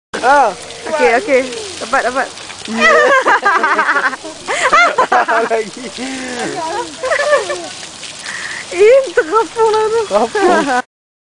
Hahaha!
Elephant pooping at the National Zoo, Kuala Lumpur, Malaysia